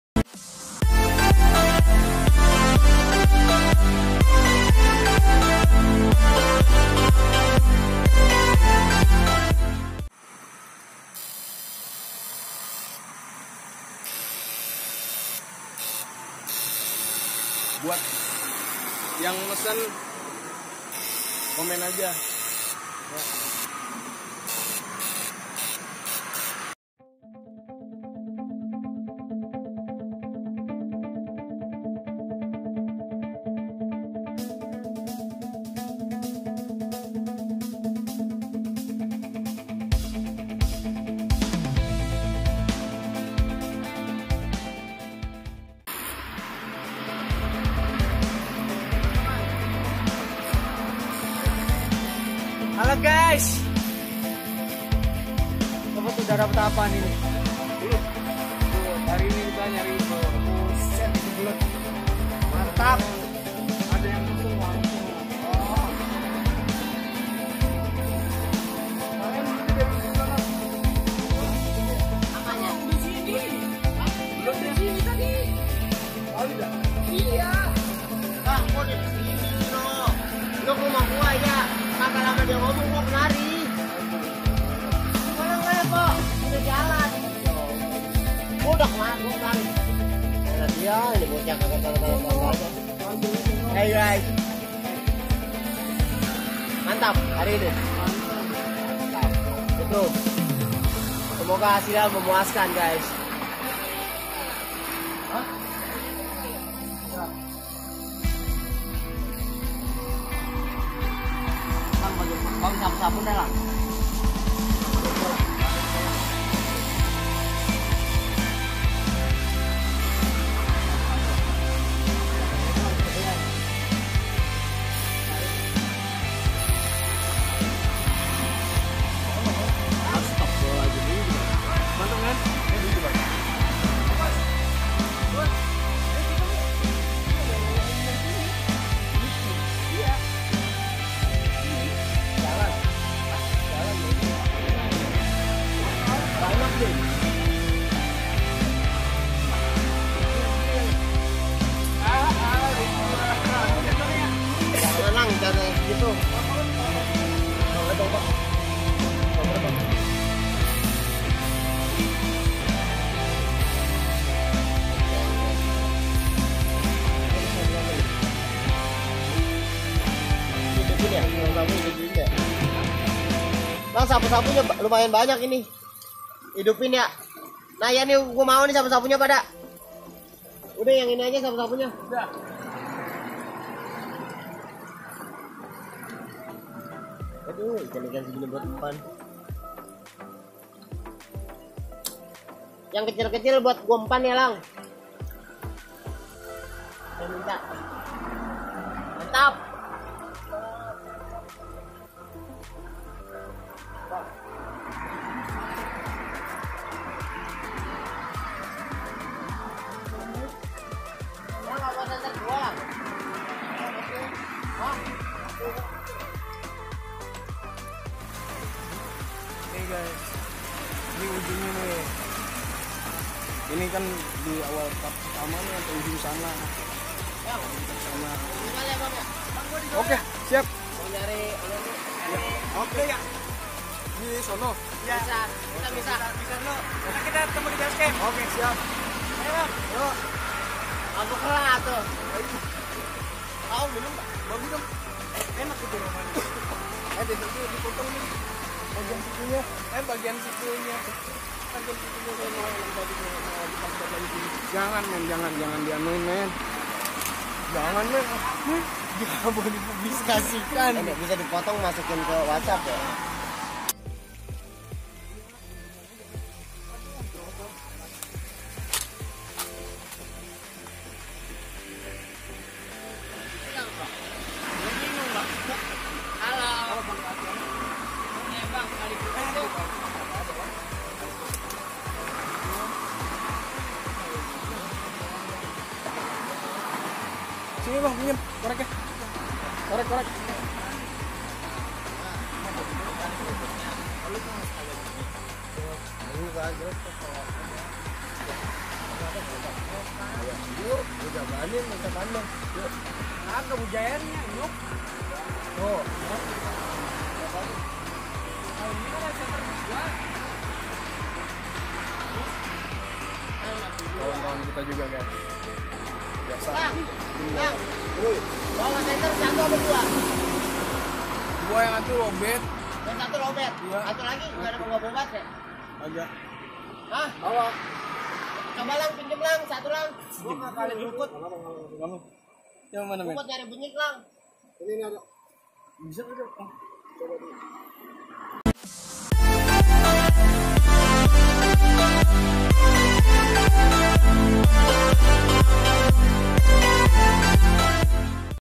berburu ikan di sungai kecil BELUTNYA BERLIMPAH || strum ikan .(part10)
hallo guys kali ini kita akan bertualang di malam hari yang terang dan indah ini. Dalam misi pencarian ikan sawah yang sudah mulai langka."

Keseruan apa malam ini. ?

Kuy di tonton vidio nya sampai habis guys..

semoga vidio kami bisa menghibur kalian.

Dan jangan lupa juga pemesanan ikan sawah langka ini..

Langsung chat aja ya guys...

Gazz keun...